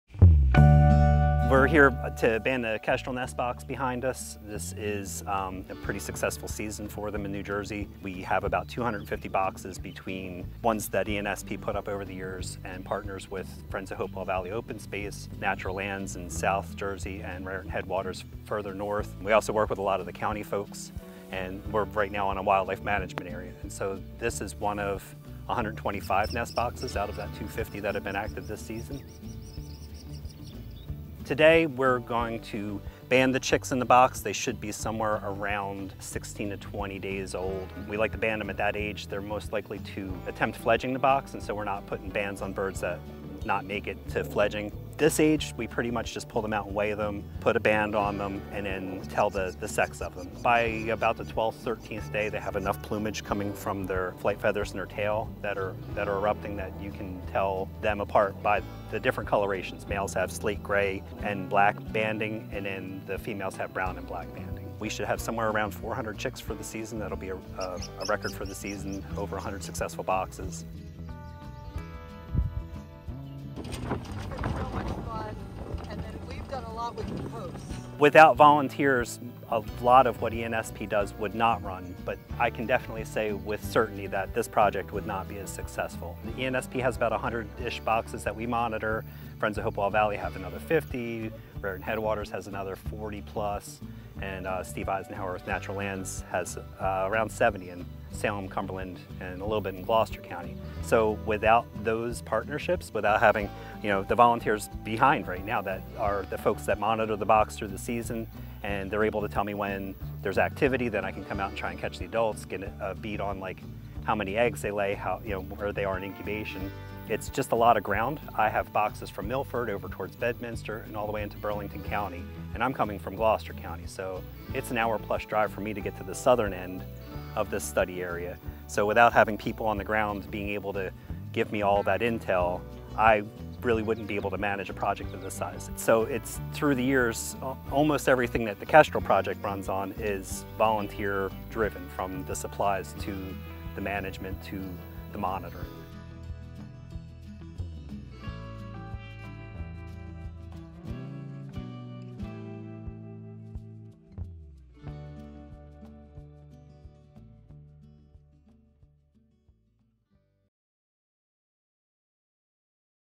American Kestrel Banding
The American kestrel is the smallest falcon in North America. Their habitat consists of open fields, farmlands, and pastures with short vegetation. They are a threatened species in New Jersey and their numbers in the eastern US have been declining for 15 years, especially in the northeast. Kestrels are cavity-nesters and accept man-made structures for nesting. Endangered and Nongame Species staff band kestrels to measure survival and population demographics, and monitor occupancy and success of nest boxes across the state.

This year marks the 50th anniversary of the NJ Endangered and Nongame Species Conservation Act, which established laws to protect and restore endangered and threatened wildlife in New Jersey. As a result of the legislation, NJDEP Fish & Wildlife established the Endangered and Nongame Species Program to carry out the work necessary to restore and maintain these species.